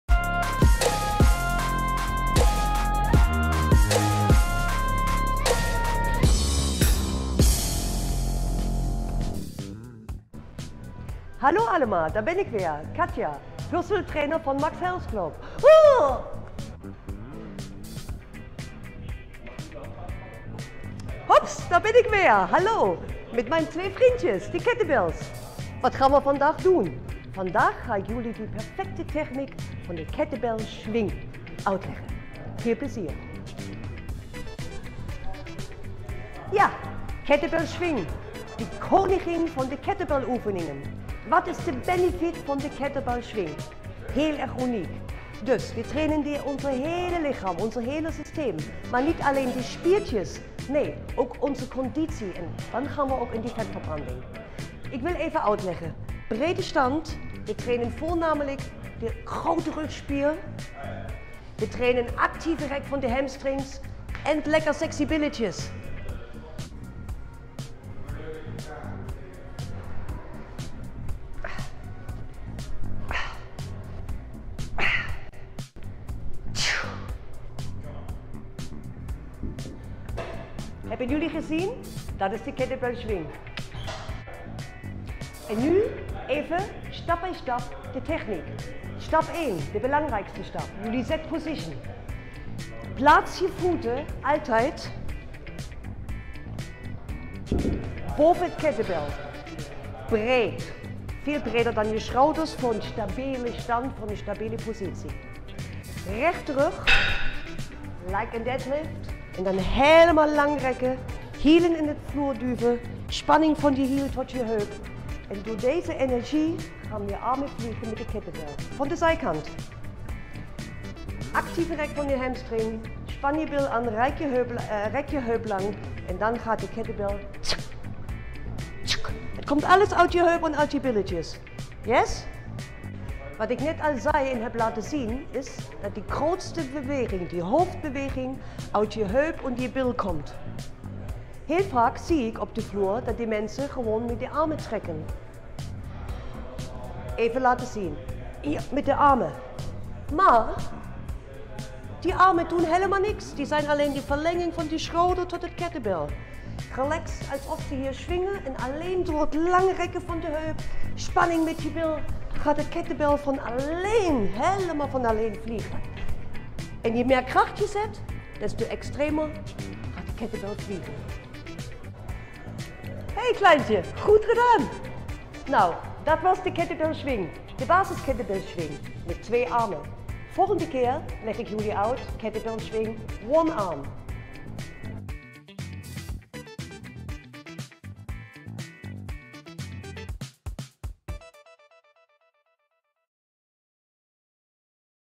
Tutorial How to do the Kettlebell Swing
Dansen in de kroeg of clubs zit er voorlopig niet in, maar swingen met de kettlebell kan natuurlijk wel! Gold Trainer Katja laat haar ‘danskunstje’ zien met een tutorial over de juiste technieken voor deze beweging. Enne, wie weet ga je straks zelf voor goud op de bovenste verdieping van de club. Gaan met die banaan!

Fitnessen bij de health club van Den Haag? voor meer informatie.